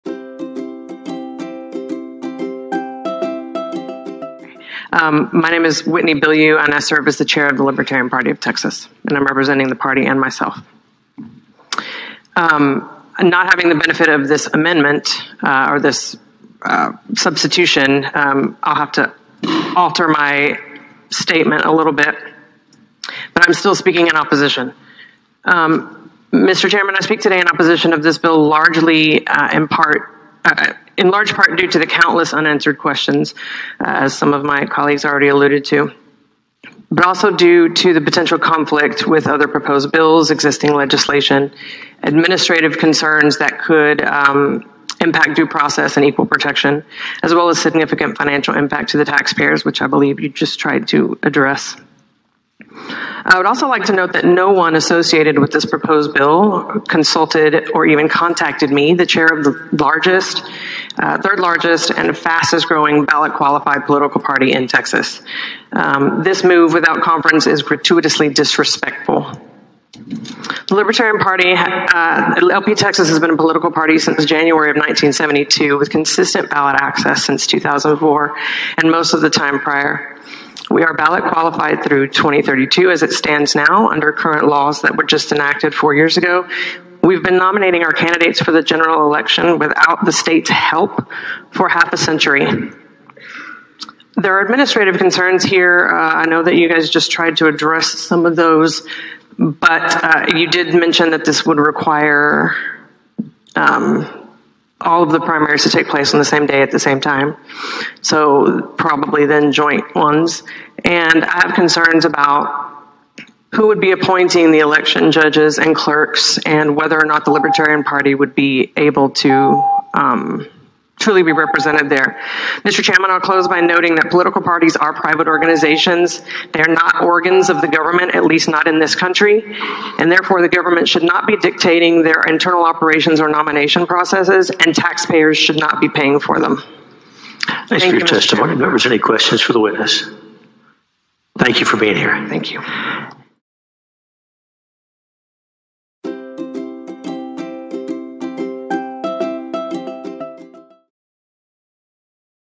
Chair Whitney Bilyeu testifies
LPTexas Chair, Whitney Bilyeu testified in the Texas Senate against S.B. 1705.
To learn more about the Libertarian Party of Texas visit LPTexas.org